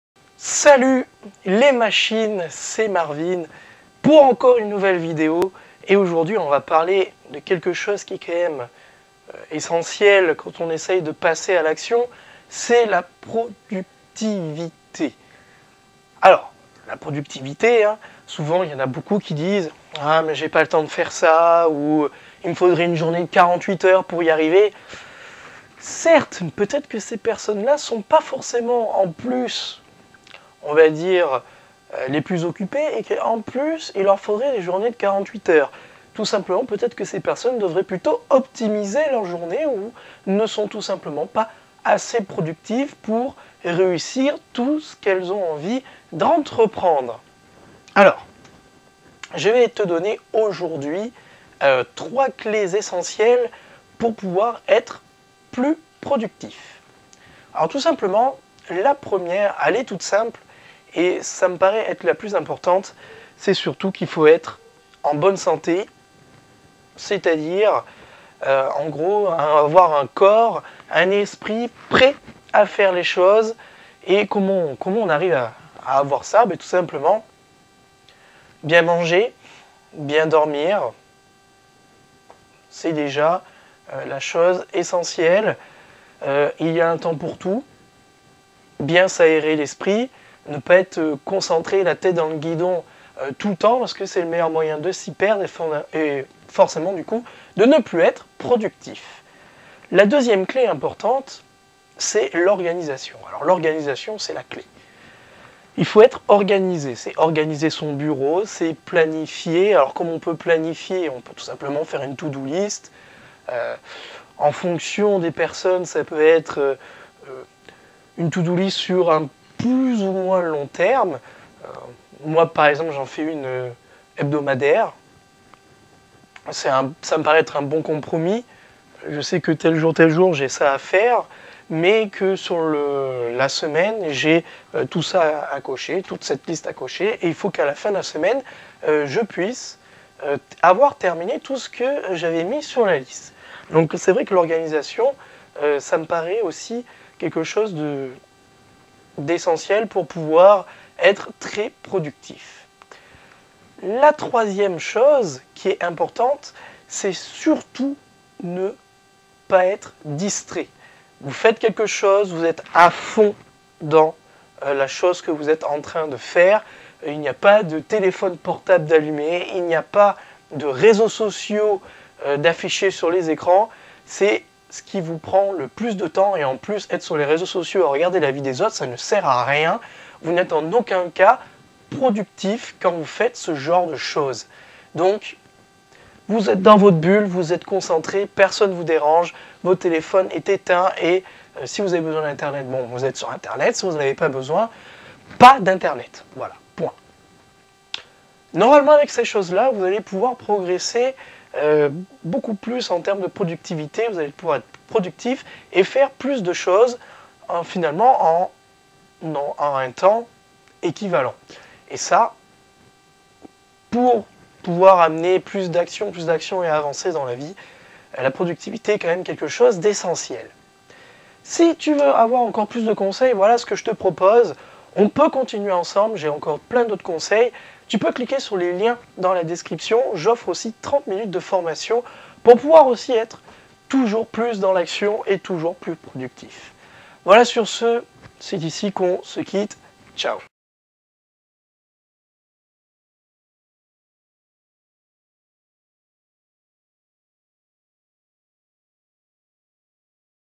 Comment être plus productif ? 3 clefs importantes.
Comment être plus productif ? Voici quelques clefs qui vous permettront d'optimiser votre temps.